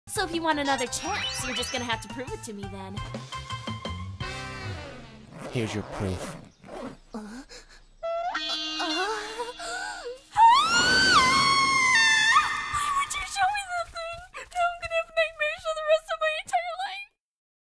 [MMD] Len Proves he's a man (Dubbed)
The Dubbed version from the 'Prove you're a man' meme made in MMD
-Len's gonna show his curious roommate, Meiko what makes him a man x3-
Motions by Crackerbarrel1000

-It's just that i remember watching this anime and that scene was a really large meme at the time xD